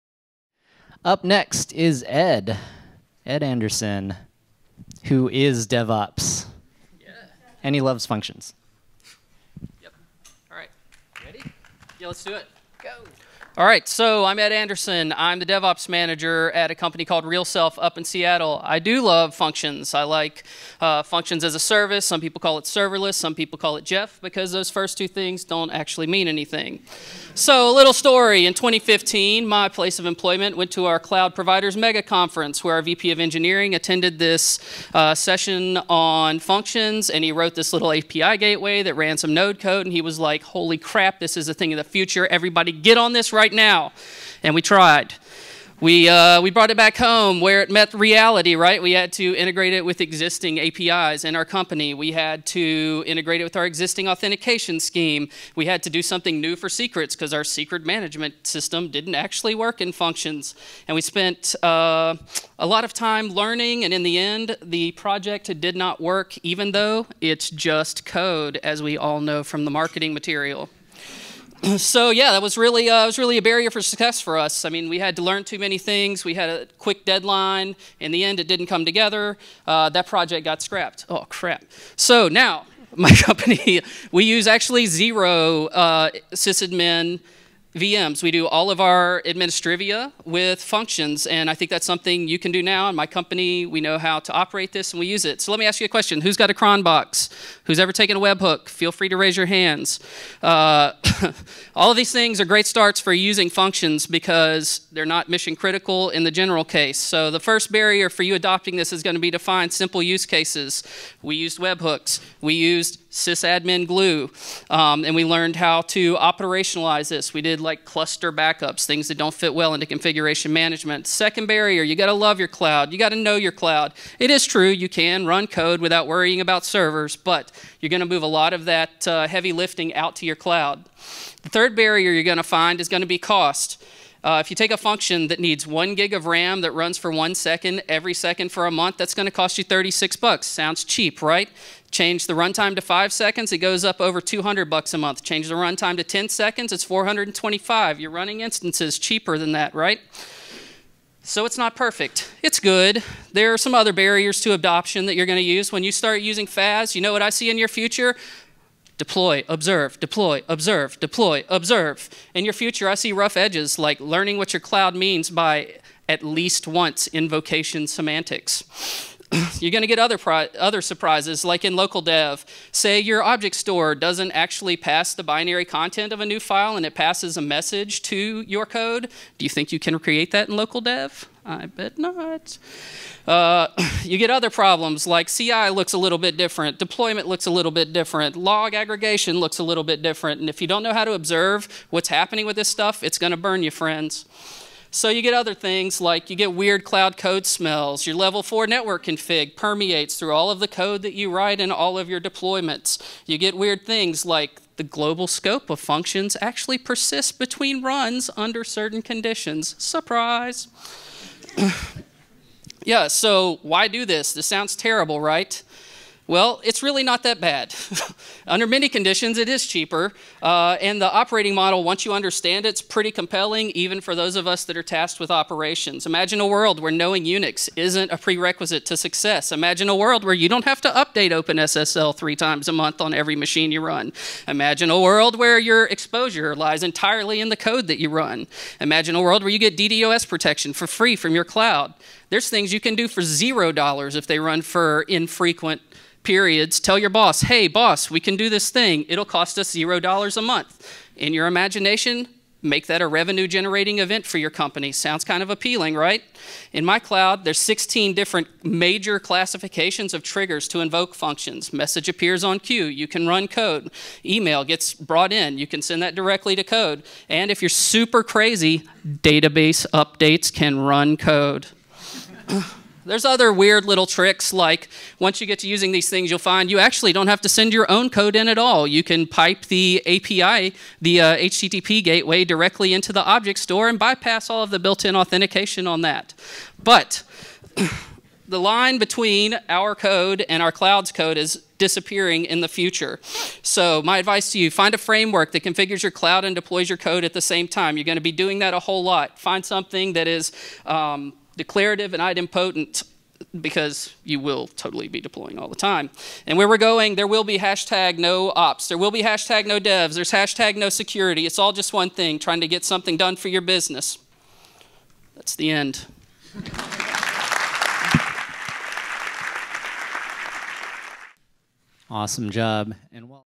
DevOpsDays Portland, 2017: Ed Anderson - I AM DEVOPS AND I LIKE FUNCTIONS
I’ve been using functions as a service ( AWS Lambda ) for the past several months, and it’s led to a very nice flow for devops administrivia.

Even though serverless is a terrible description, and those no-ops people are crazy, functions as a service is still a super interesting approach, especially if you’re in the devops world. So many things that we need to accomplish are event driven, and we can lower both our capital costs, and our security exposures by using FaaS to accomplish them ( provided that you don’t give cloud-admin privileges to your Functions ).

Aerial Video:
Didacus Games (YouTube)